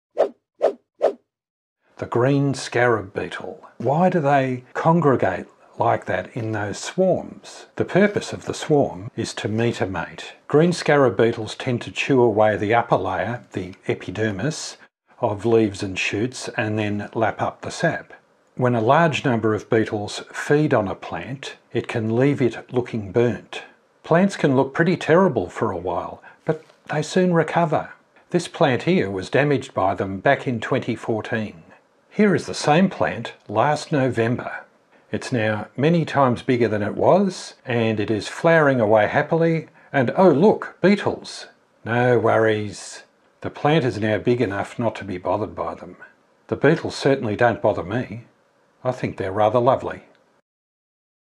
Aussie green scarab beetles seem like pests. But are they?
Green scarab beetles (Diphucephala colaspidoides) are regular visitors at my place. They are native insects common in SE Australia.